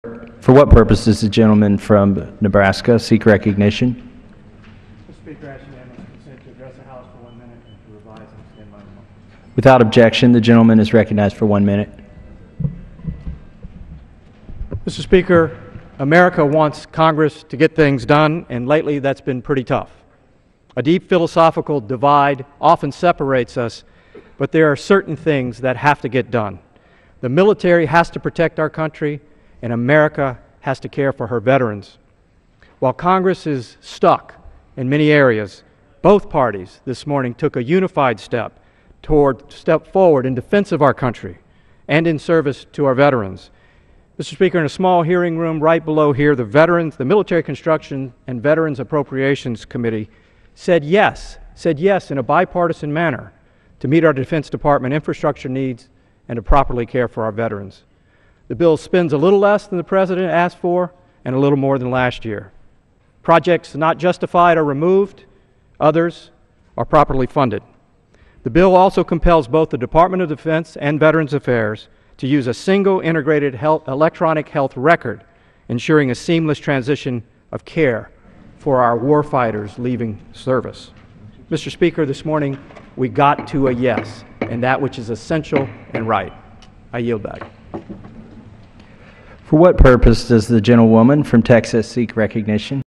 Fortenberry Floor Speech on Bipartisan Cooperation to Help Our Veterans
Congressman Fortenberry gives a "one minute" speech on the House floor on bipartisan cooperation to help our nation's veterans.